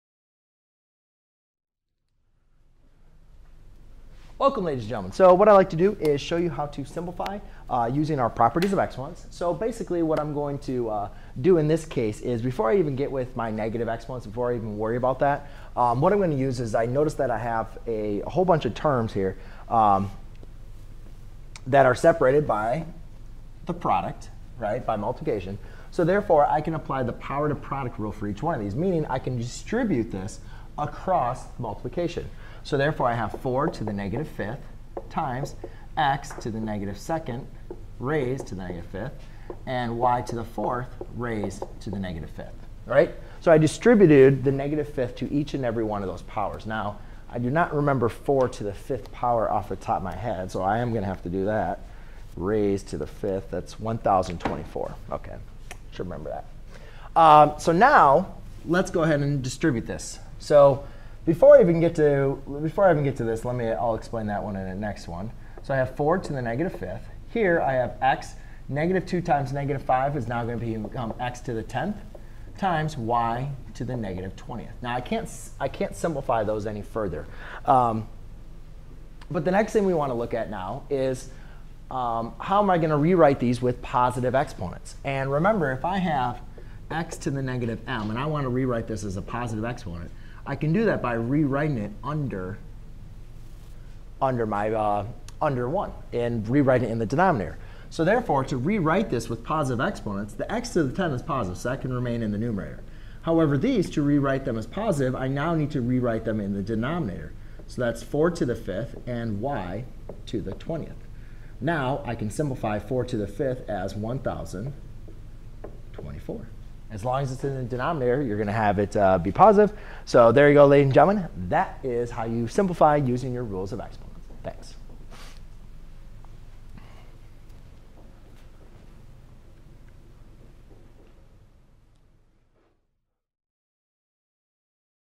Applying multiple rules of exponents to simplify an expressions
👉 Learn how to simplify expressions using the power rule and the negative exponent rule of exponents. When several terms of an expression is raised to an exponent outside the parenthesis, the exponent is distributed over the individual terms in the expression and the exponent outside the parenthesis is multiplied to each of the exponents of the individual terms of the expression.

The negative power rule of exponents states that a power with negative exponents is equivalent to the reciprocal of the power with the positive exponent.